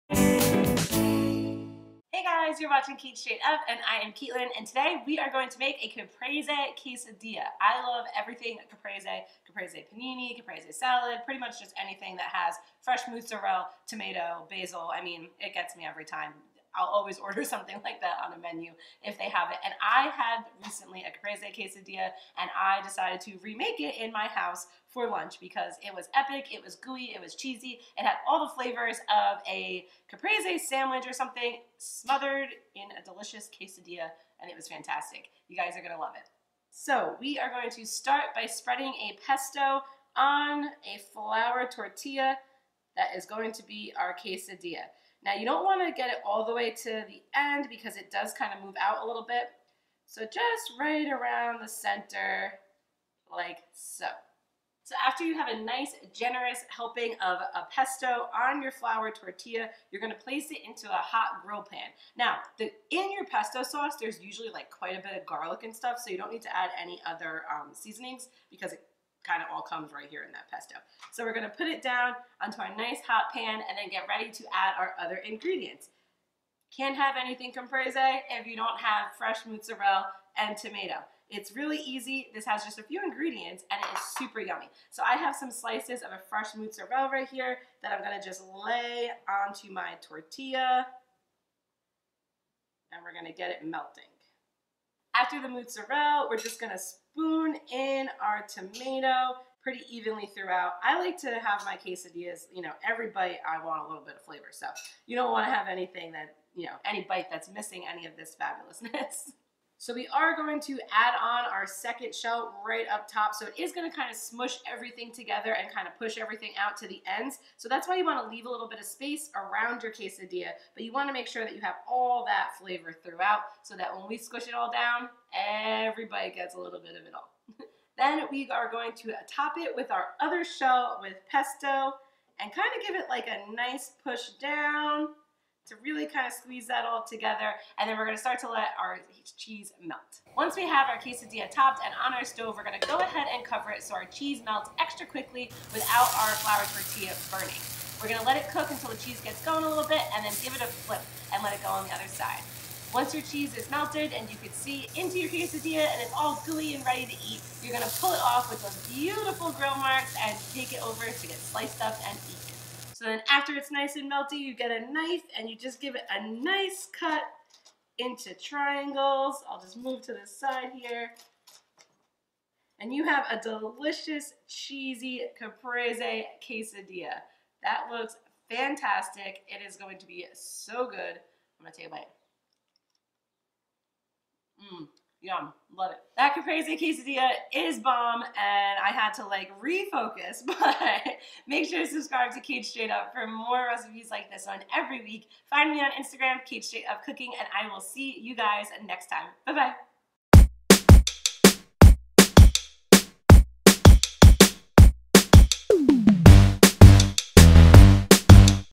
Caprese Quesadilla Recipe | Cait Straight Up
I love a good quesadilla and I love anything caprese - caprese salad, panini's - you name it! So why not make a Caprese Quesadilla?! This recipe combines all of my favorites - tomato, melty fresh mozzarella and a garlic-y pesto to make a delicious homemade quesadilla. My recipe doesn't require too many ingredients (you can jazz it up if you want!) but is super tasty. It is easy to make at home and is quick enough for lunch or a snack. If you love caprese anything or just the idea of melting fresh mozzarella, tomato and basil pesto together in a quesadilla, give this recipe a try!

Or submit a request! Cheers!

Enjoy these dillas!!